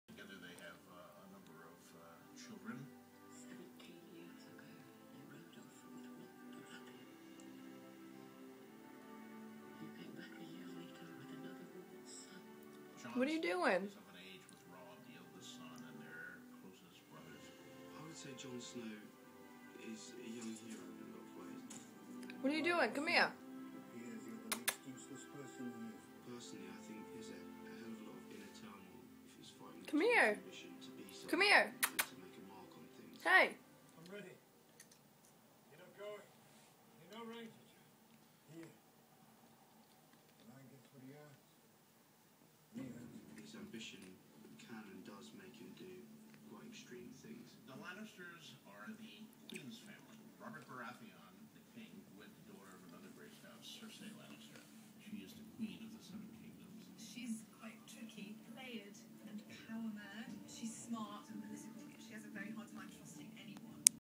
Carlos the chihuahua walking with cramp funny
Carlos must have had cramps and tried walking them off